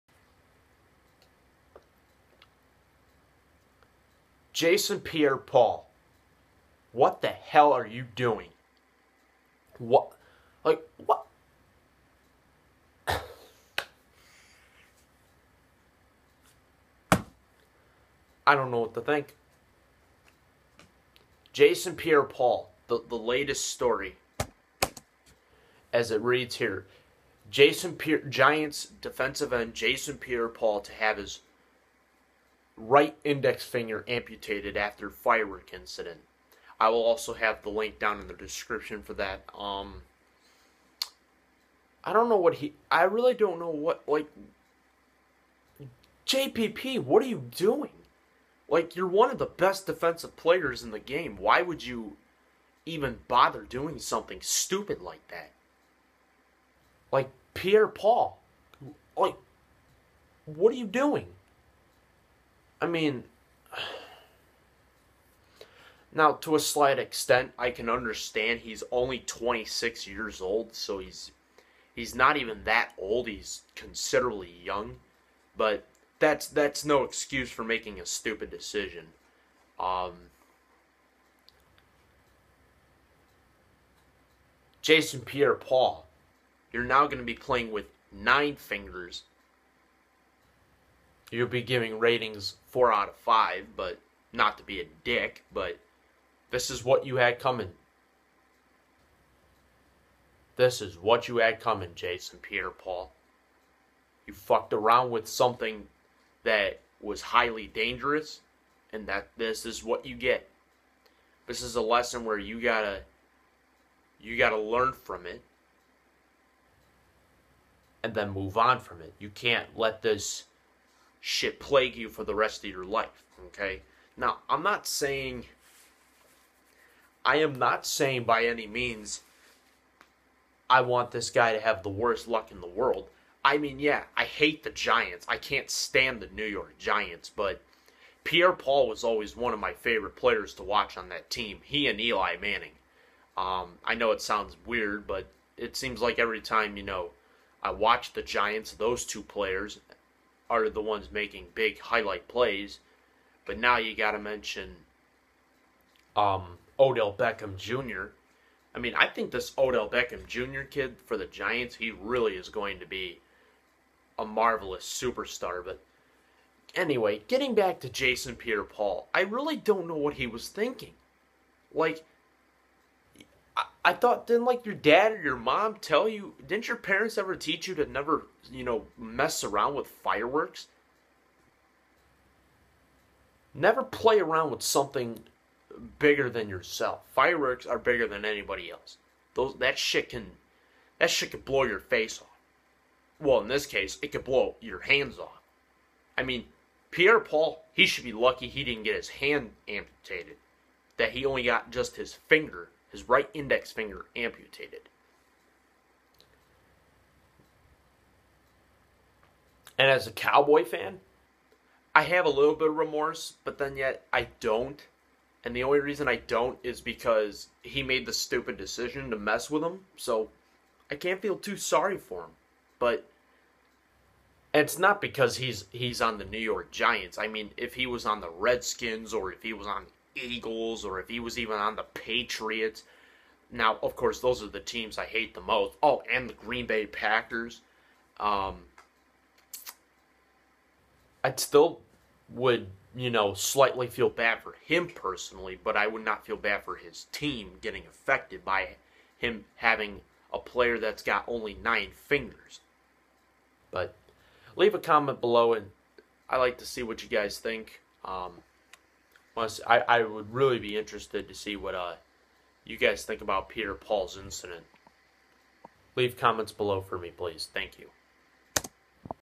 Jason Pierre-Paul Fireworks Incident UPDATE* VLOG #15
New York Giants DE Jason Pierre-Paul has finger amputated but is still expected to play in 2015. My thoughts and opinions in the video as well.